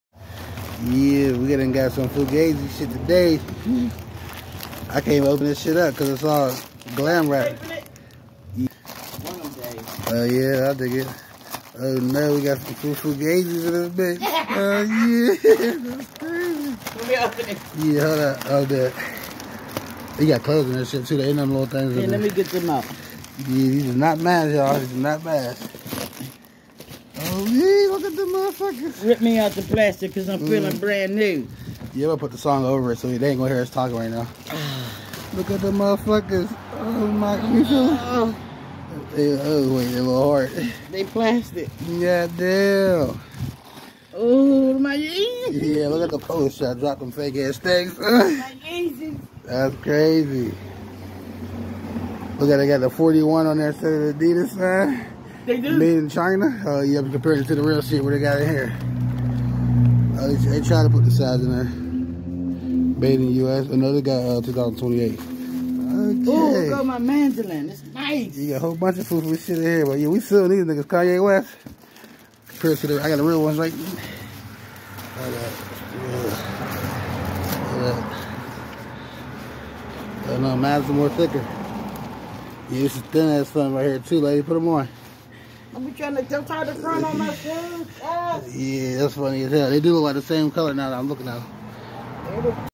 Yeezy Foam Runner "Vermillion" Full Unboxing
FAKE TEMU $8 YEEZY FOAM RUNNER FULL UNBOXING & COMPARISON